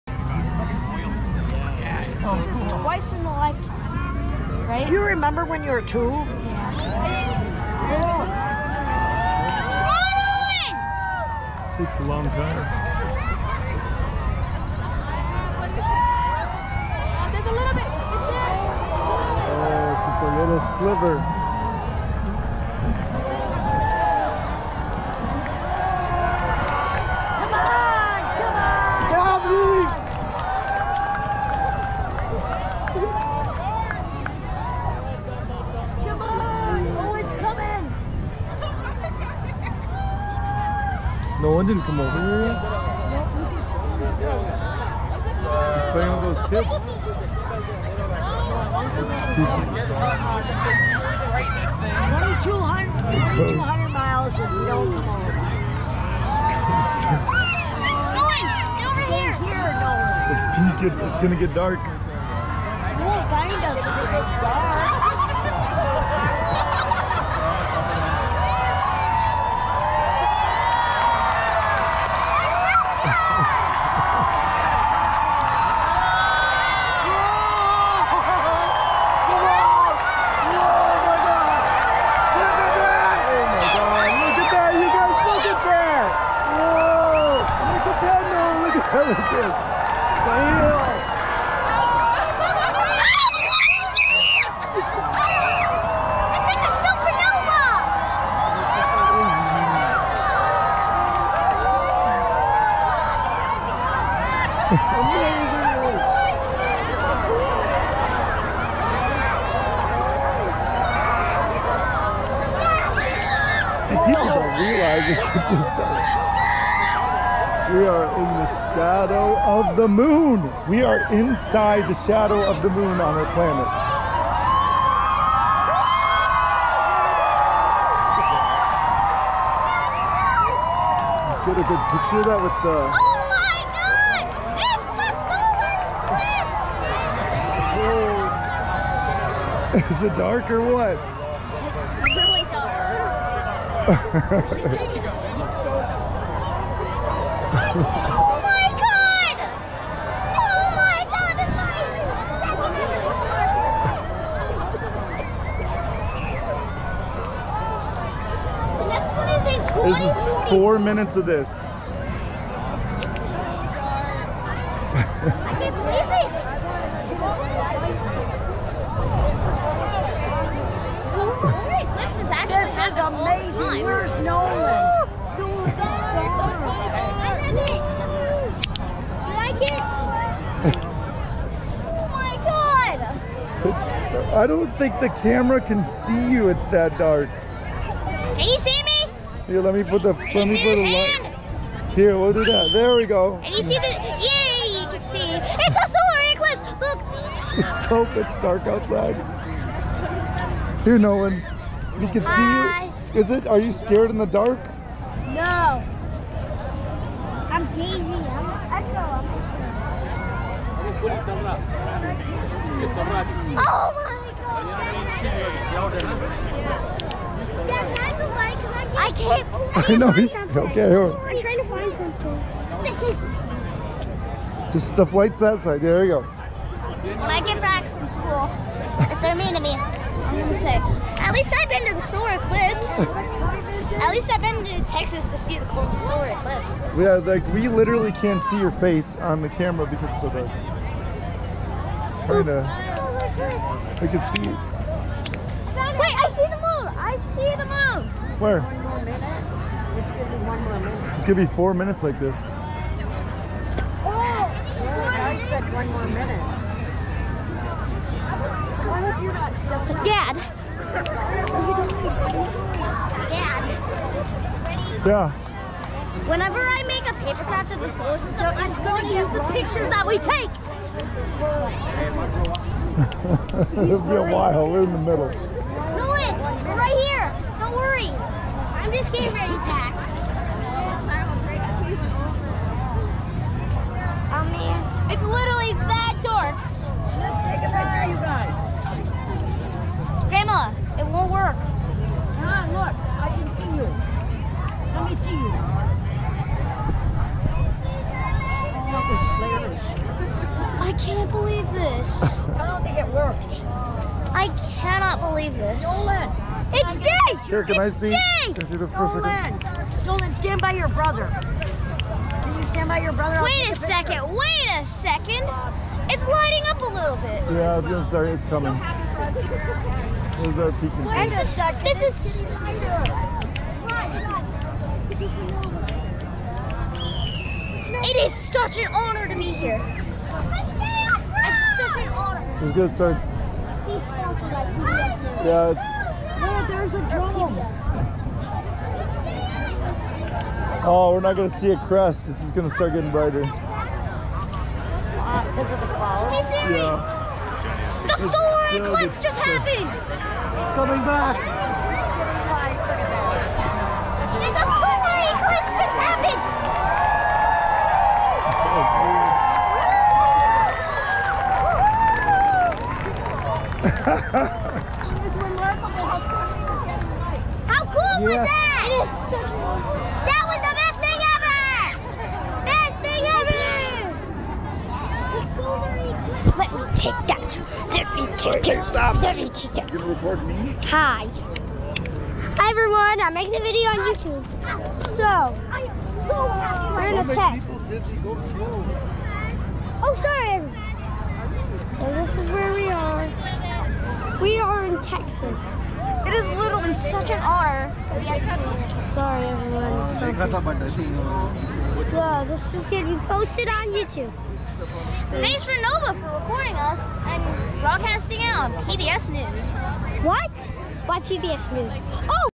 Solar Eclipse April 8 2024 Kerryville Texas Totality 4K
Words alone cannot describe the magnitude and excitement of this rare, once in a lifetime event. Lucas has seen both 2024 and 2017 as he mentions in the video. There little comparsion between partial and total eclipses. It dosent require a love for sicence or space to appreciate the indescribable experience